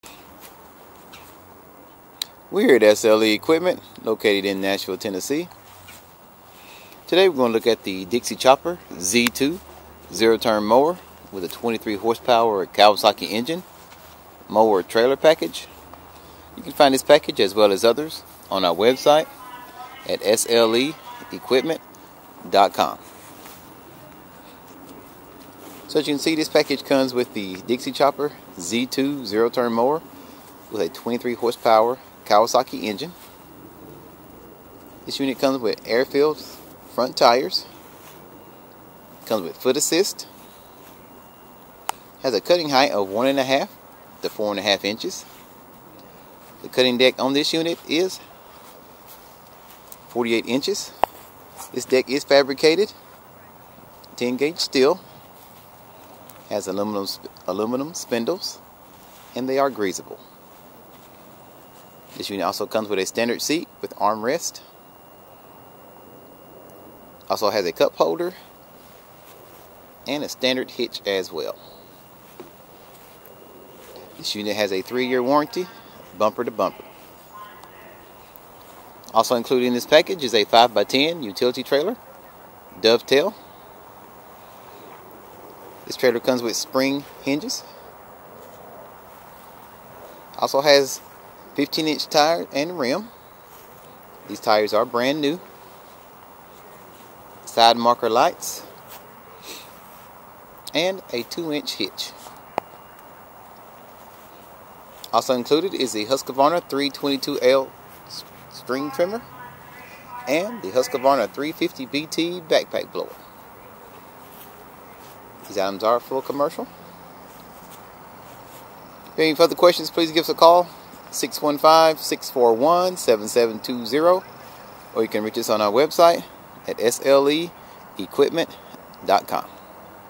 Dixie Chopper Zee 2 2348 Kawasaki Handheld Trailer Package
This Package Includes :
Dixie Chopper 2348 Zero Turn
Husqvarna 322L Straight Shaft String Trimmer
Husqvarna 350BT Backpack Leaf Blower
Utility Trailer 5' x 10' Dove Tail With gate

Hours
Sunday: closed

Nashville Location
3533 Murfreesboro Pike